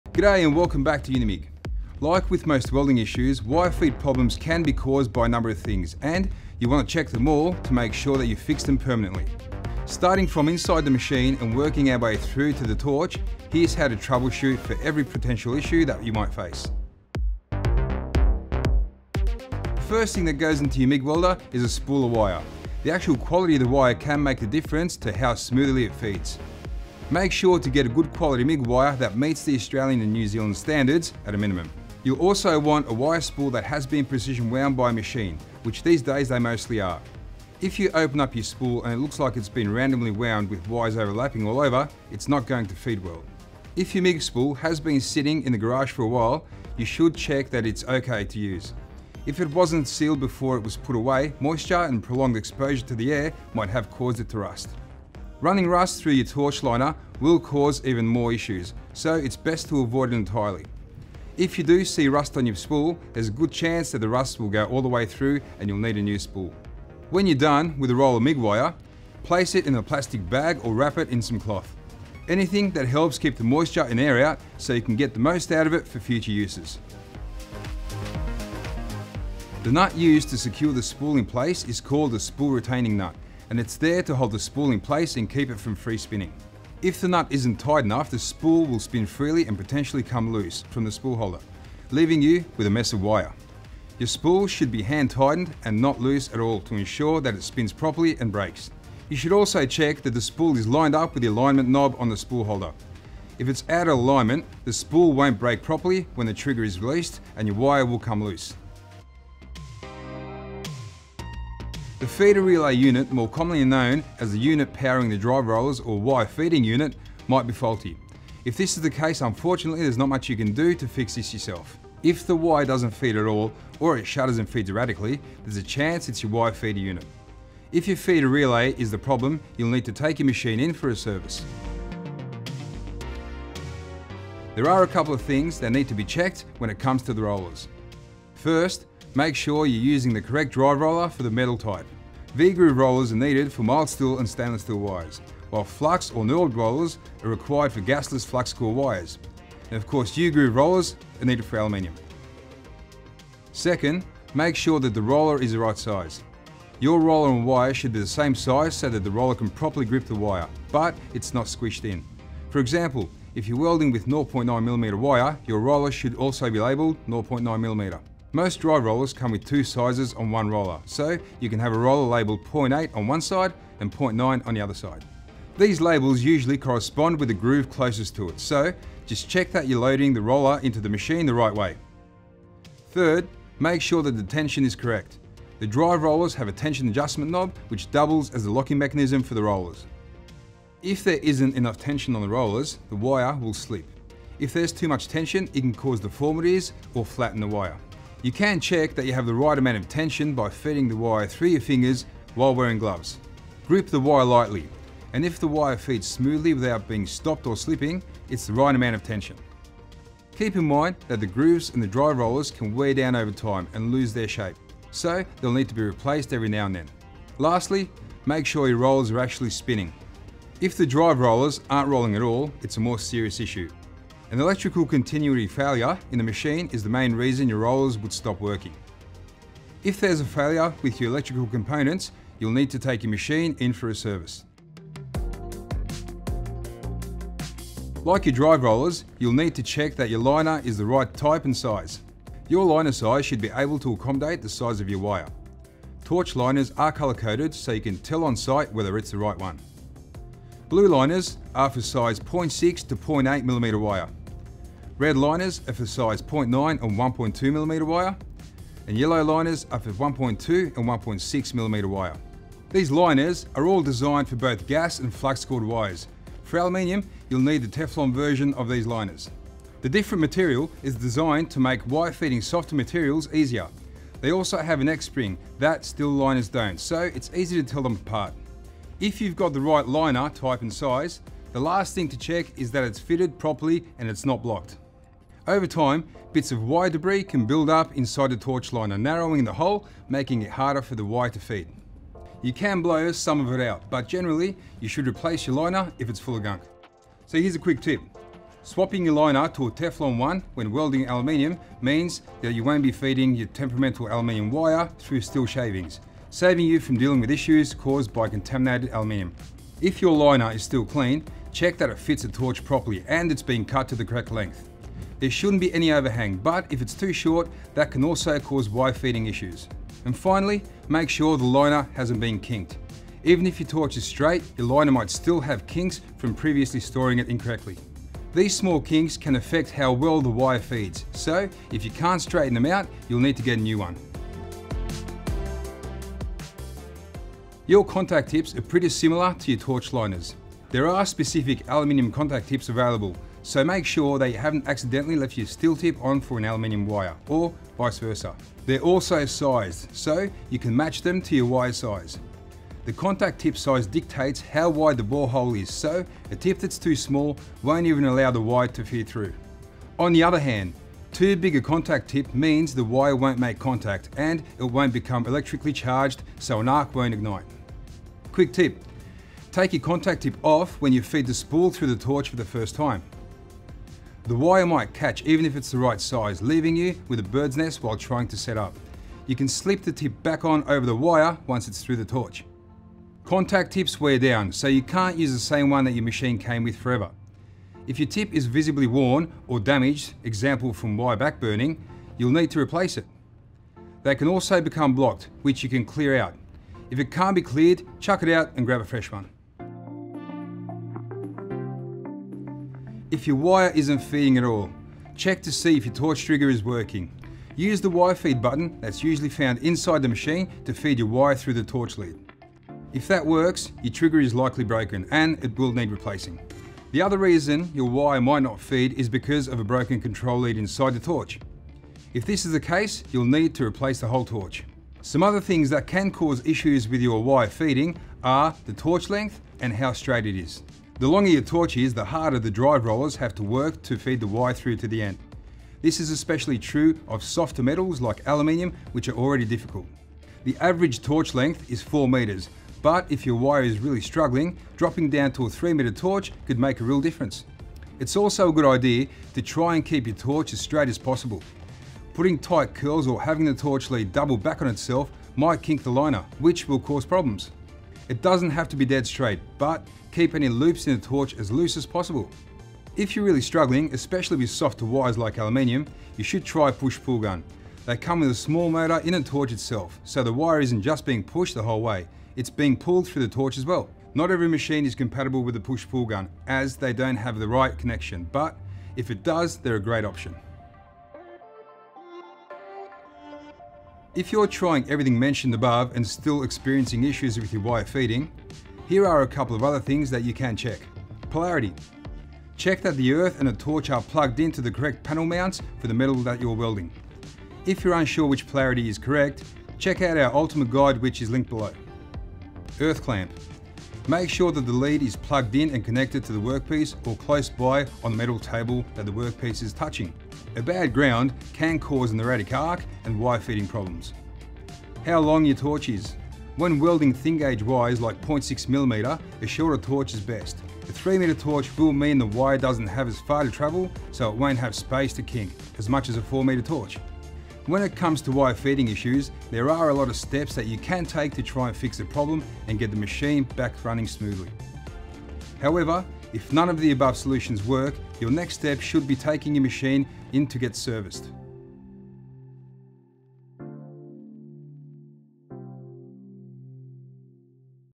Troubleshooting MIG Wire Feeding Issues in Your Welder
Have you ever had a wire feeding issue with your MIG welder? If you have, you'll know just how many things can go wrong. From the wrong liner to a faulty drive roller system, any point in the welder or the torch could be causing problems. It can make troubleshooting a long and drawn out process. We've gone through and covered all the possible problems that might cause wire feeding issues in your machine, so the next time you need to troubleshoot, you'll have a better idea of what to look for.

⏰ Timecodes ⏰
0:00 - Intro
0:17 - Good Quality Wire?
1:17 - Is the Spool Loaded in Properly?
1:49 - Check the Wire Feeding Unit
2:12 - Drive Rollers
3:55 - Liner
5:34 - Contact Tip
6:39 - Torch Leads and Trigger
8:01 - Other Things to Check